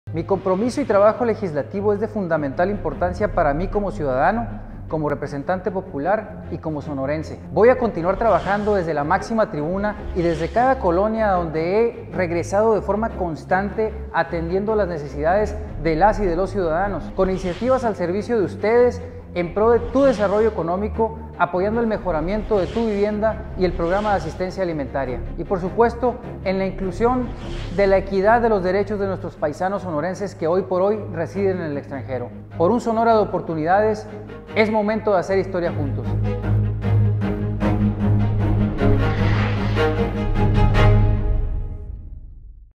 Primer Informe Legislativo II: Kiko Munro
Primer informe de trabajo legislativo con leyes al servicio de la gente: Kiko Munro

-          Trabajamos en dotar a la gente de leyes funcionales y sobre todo, leyes a su servicio que realmente solucionen sus problemáticas de la vida diaria en todos los ámbitos.